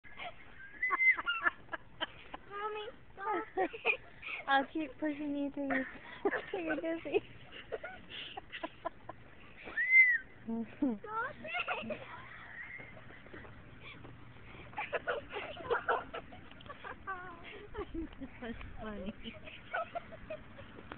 Our daughter having fun
Our daughter having fun on the playground